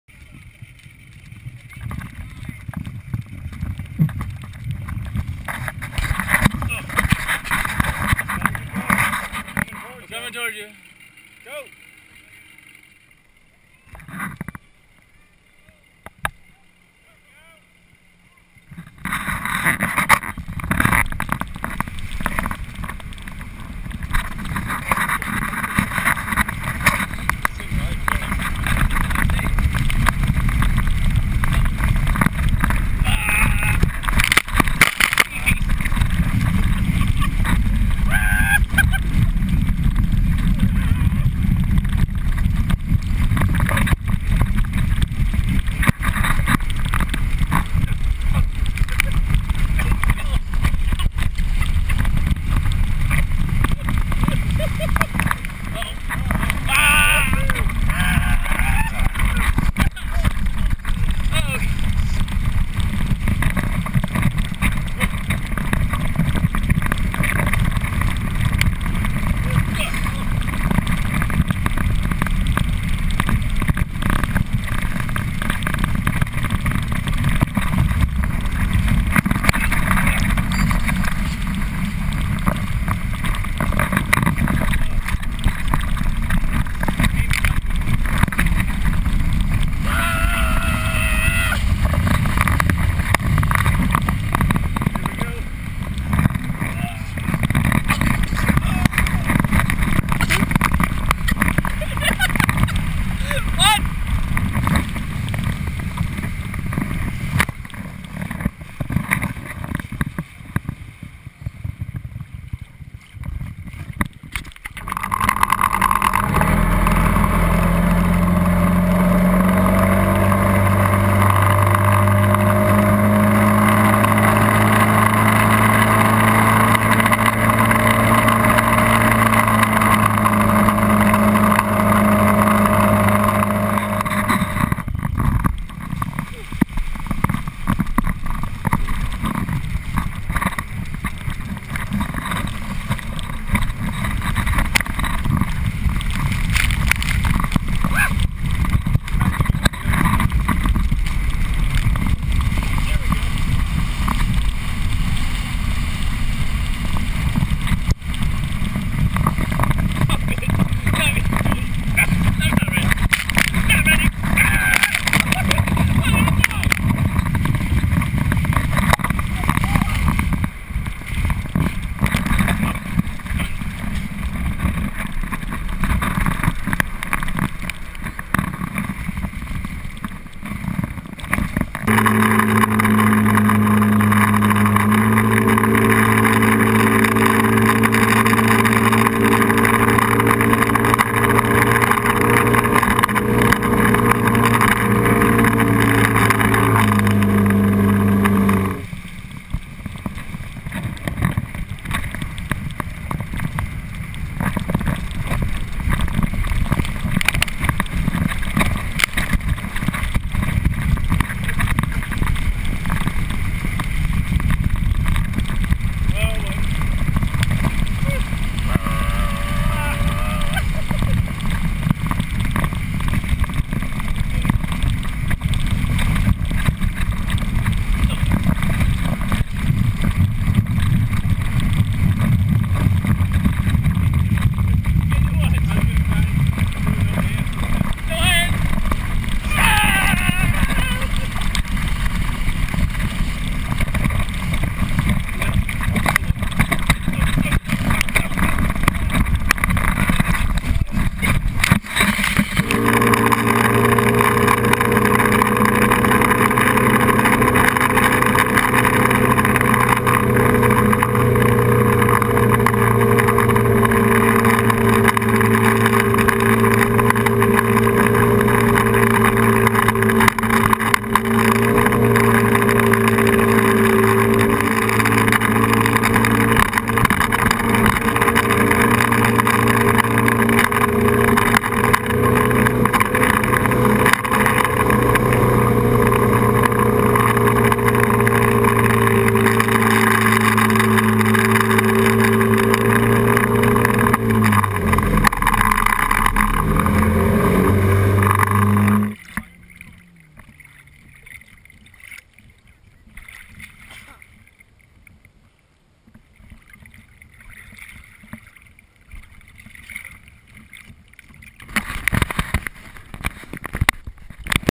GoPro HD HERO camera: Extreme POV Water Sports
With the GoPro HD Hero, you can get great camera shots from just about any angle. The mounting system is versatile enough to mount it anywhere from on your head to on the handle of a tube you're ridin